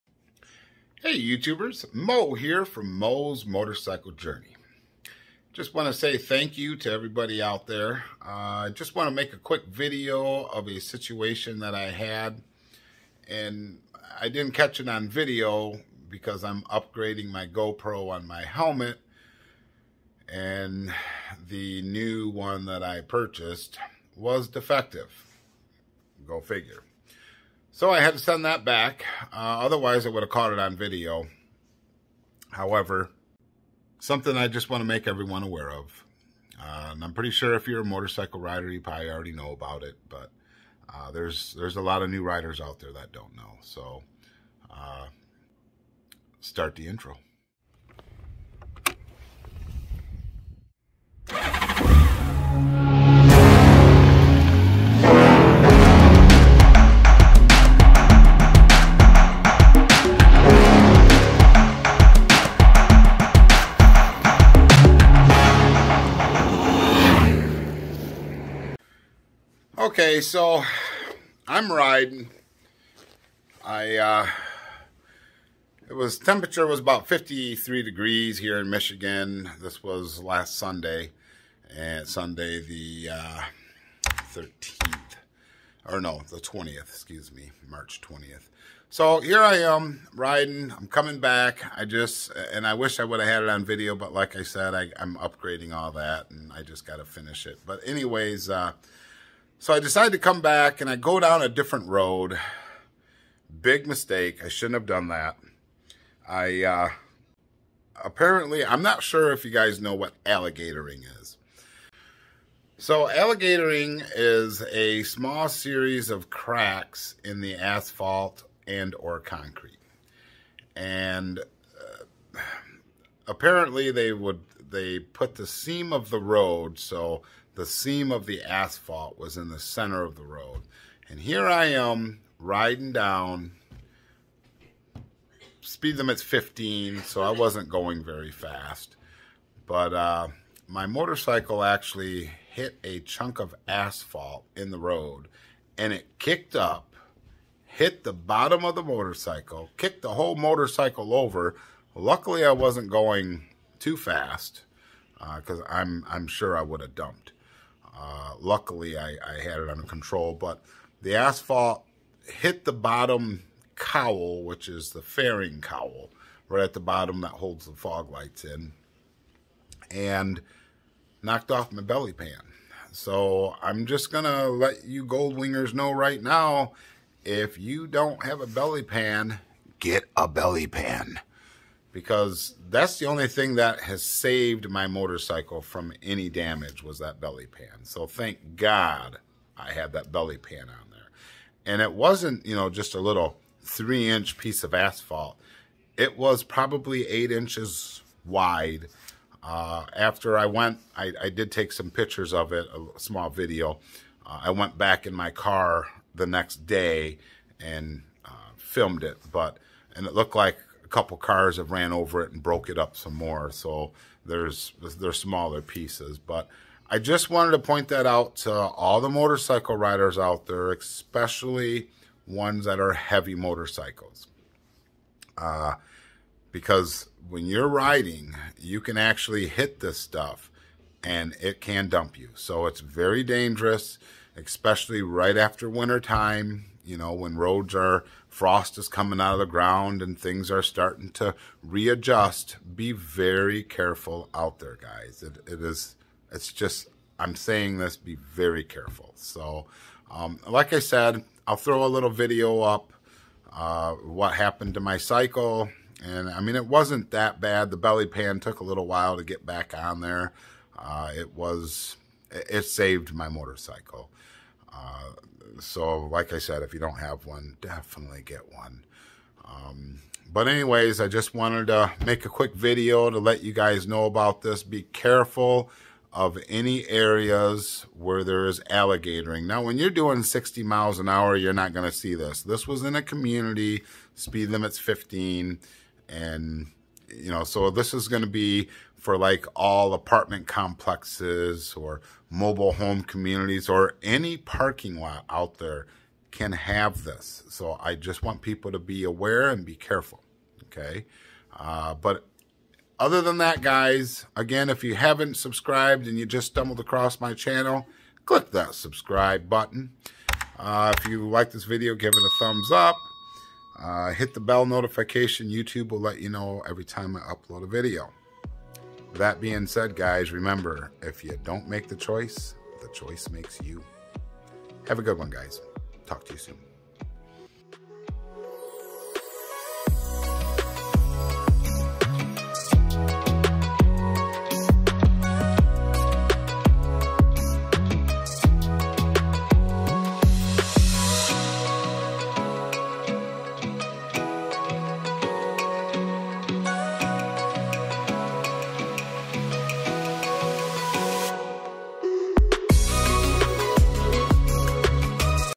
I CANT BELIEVE THIS ALMOST DUMPED MY GL1800 GOLDWING.
IN THIS VIDEO I HOW HIT PART OF AN AREA IN THE ROAD AND KICKED UP A HUGE PIECE OF ASPHALT CAUSING ME TO ALMOST DUMP MY GOLDWING, SCRATCHING THE LOWER FAIRING COWEL AND KNOCKING OFF MY BELLY PAN.